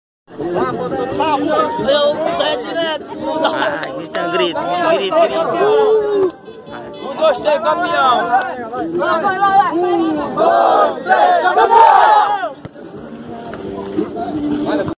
Internacional de Aroeiras Campeão do Torneio de Julho em Aroeiras
Internacional de Aroeiras 2X1 Palma de Coreaú. Gols do Wilton Rodrigues e Duliano Parente. -

Todos os vídeos são filmados, de propriedade e gravados por Aroeiras Amadores. Eu adquiri todos os direitos do conteúdo publicado.